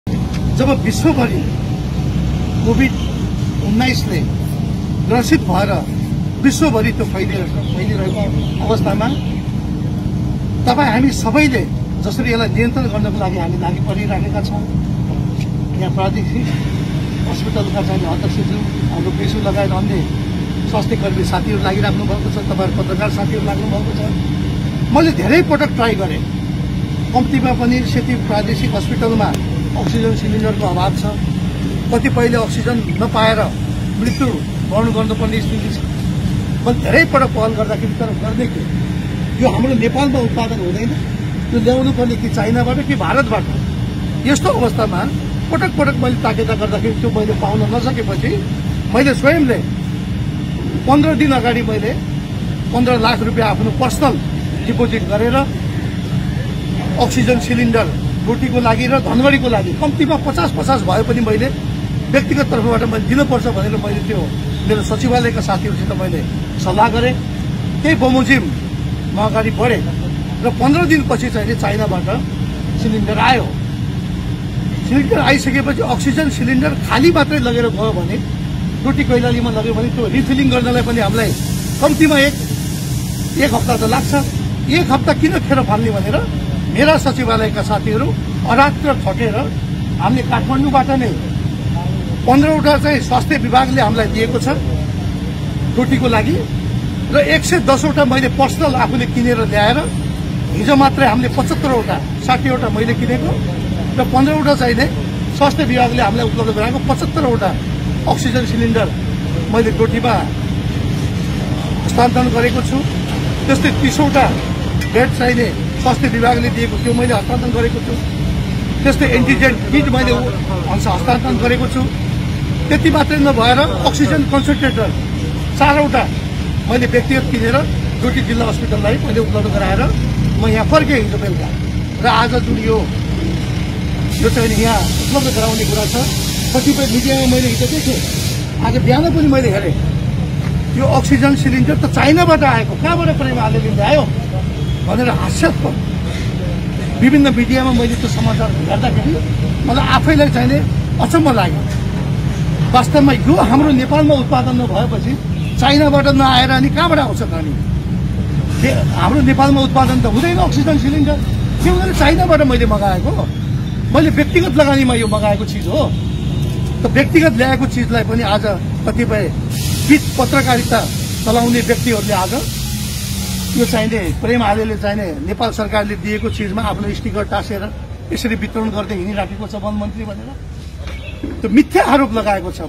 अक्सिजनकै कारण फेरि चर्चामा वनमन्त्री, स्टिकर टाँसिएको सिलिन्डर चिनियाँ सहयोगकै हो? वनमन्त्रीको जवाफ
अक्सिजनकै कारण फेरि चर्चामा वनमन्त्री, स्टिकर टाँसिएको सिलिन्डर चिनियाँ सहयोगकै हो? यस्तो छ आलेको जवाफ